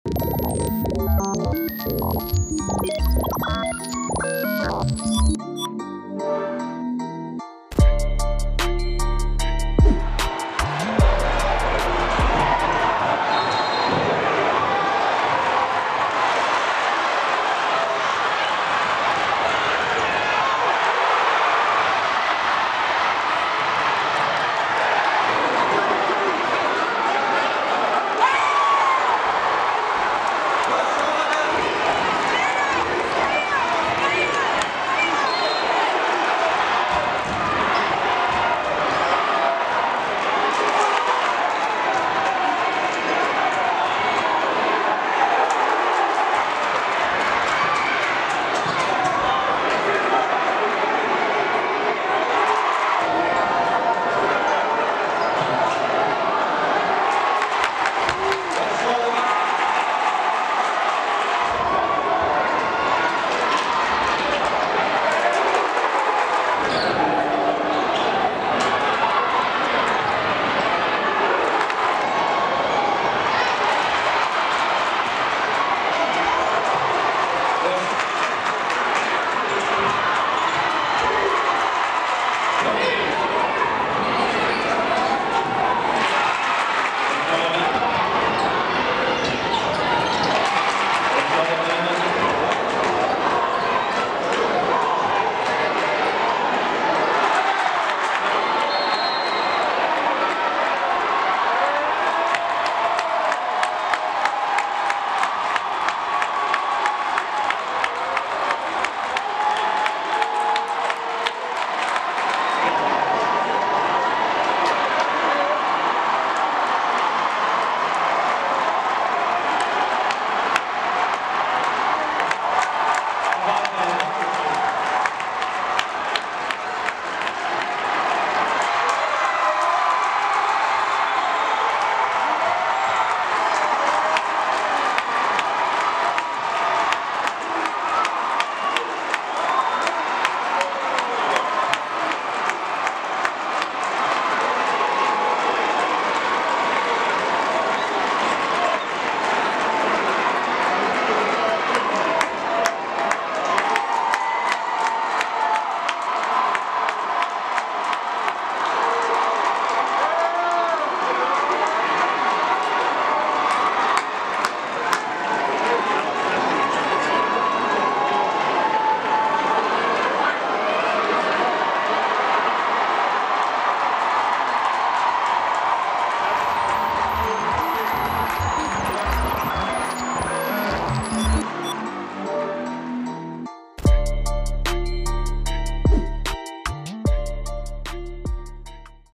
Day'Ron Sharpe & South Central: Best Team in NC History?! 4A State Champions CROWNED!!
NCHSAA STATE TOURNAMENT
4A State Championship
South Central High School: 72
West Charlotte High School: 46